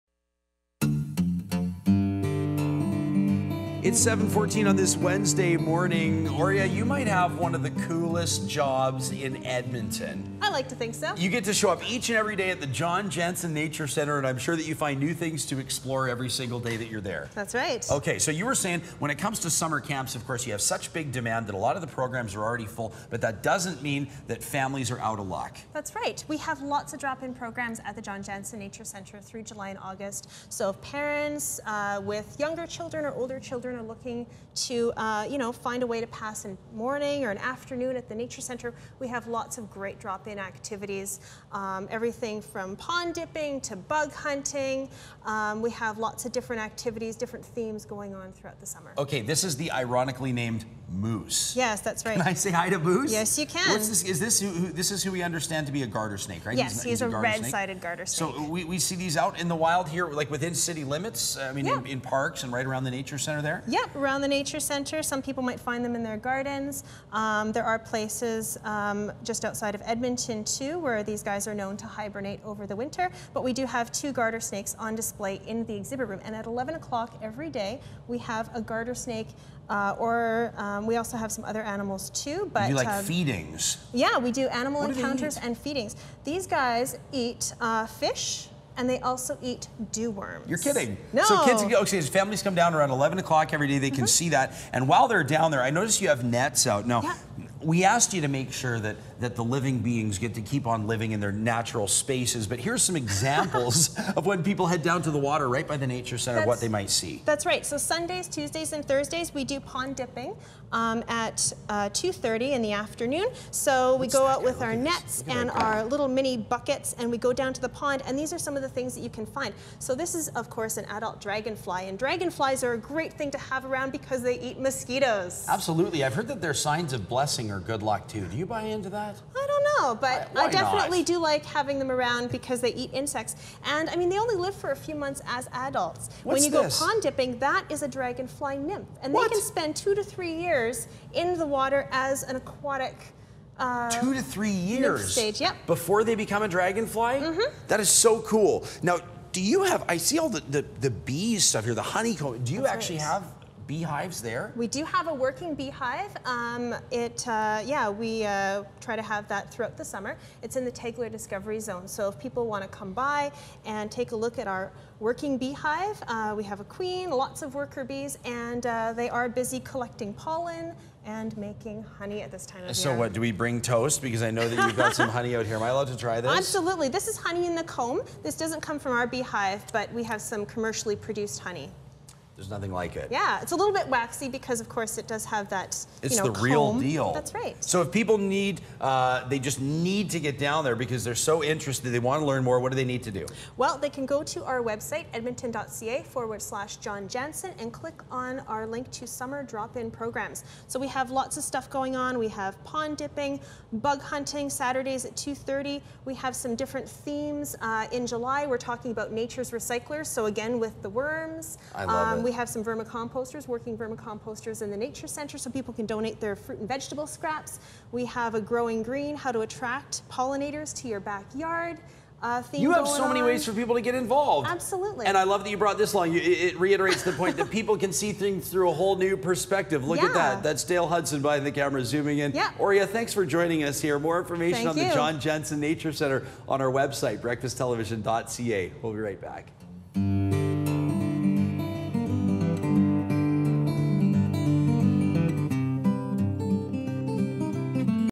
Bugology
Bonkers about bugs. We show you what all the 'buzz' is about at the John Janzen Nature Centre. This is not an exercise in being creeped out, but an opportunity to boost your bug knowledge during several summer camps and workshops this summer.